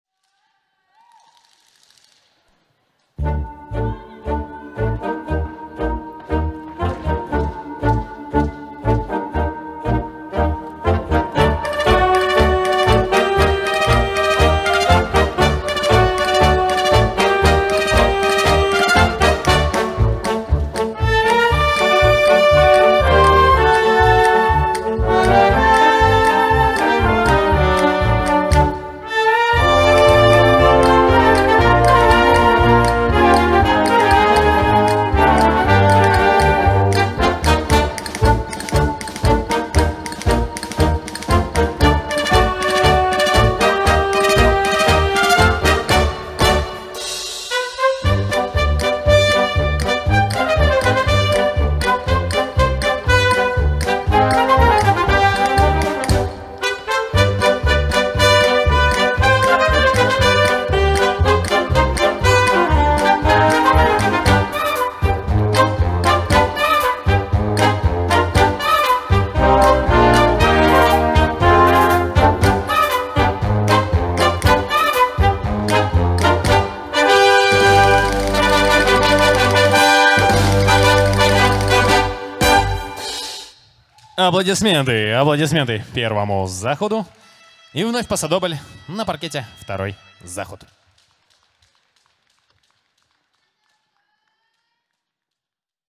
☂Молодежь 1 Rising Stars (до 19 лет)(Open) PasoDoble(PD) ПолуФинал LA(5) Capital Cup Minsk – 2021
Capital Cup Minsk – 2021 Международные соревнования по спортивным бальным танцам
дата проведения: 16-17 октября 2021 года
Организаторы:

Couple №103: Гришко Матвей и Хаткевич Мария
(Club: Sigma, Минск, Беларусь)
Trainer (s): Козыро Артём Александрович и Бабич Наталья Николаевна